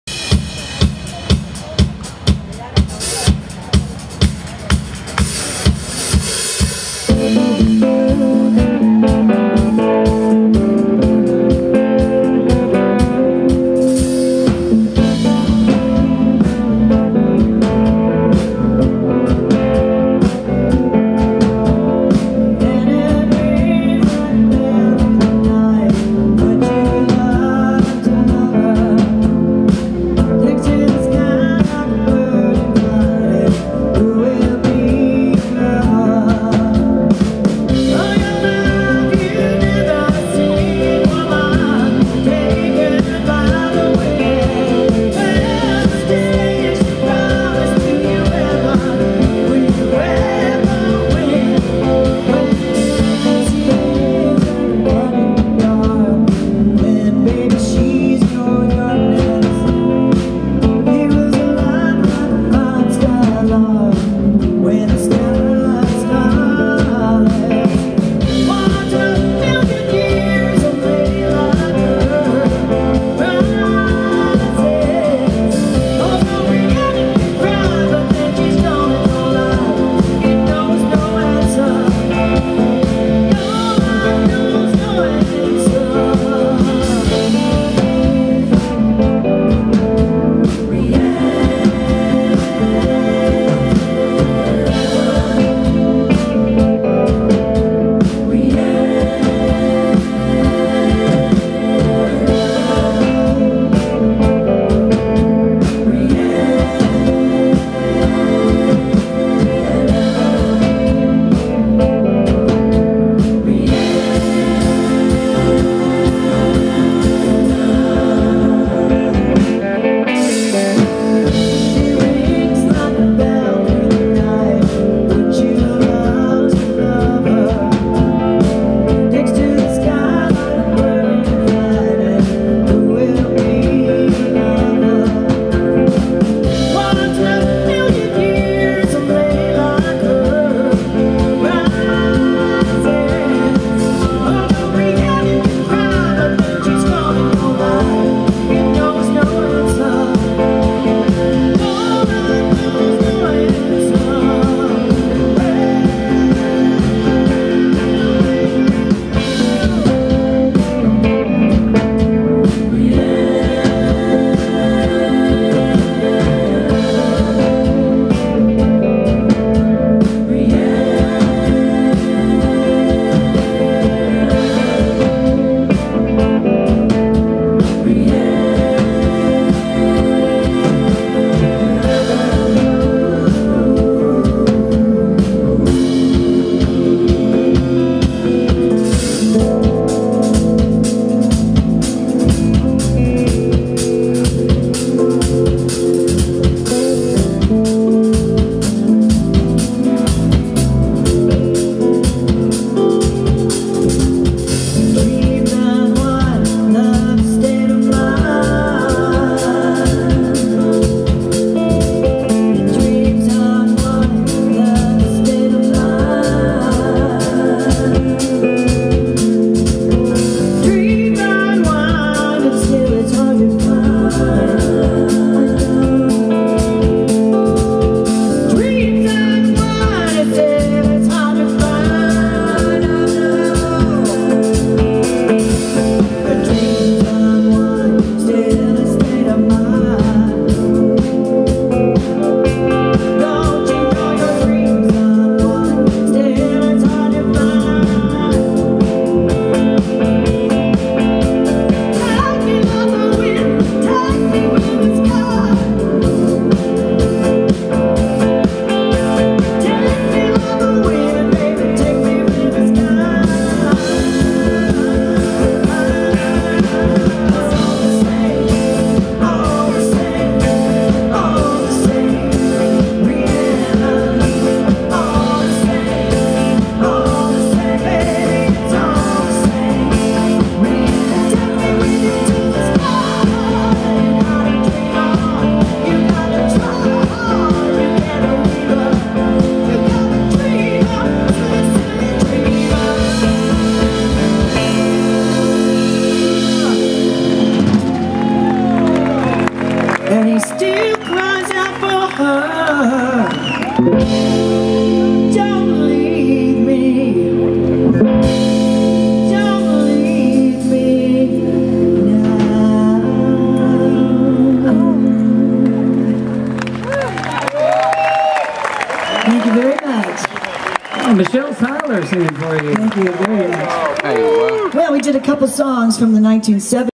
Mirage, the vision of Fleetwood Mac - Woodland Hills - 07/09/17
Description MIRAGE - Visions of Fleetwood Mac celebrates the classic lineup of the legendary superstar band. Based in Los Angeles, the members of "Mirage" capture the look and sound of Fleetwood Mac live in concert. The band is a spinoff of the highly successful band, "Bella Donna - A Tribute to Stevie Nicks", which was awarded the highest honor of being praised by the real Ms. Nicks after she heard a live performance by singer Michelle Tyler and the band. "Bella Donna" also appeared on AXS TV after being crowned one of The "World's Greatest Tribute Bands". "Mirage" focuses squarely on Fleetwood Mac and their Greatest Hits in the five piece authentic concert recreation.
Michelle Tyler (vocals) as Stevie Nicks
Bruce Lawrence (guitar, vocals) as Lindsey Buckingham
Carla Buffa (keyboards, vocals) as Christine McVie
Bob Weitz (bass) as John McVie
Richard Graham (drums) as Mick Fleetwood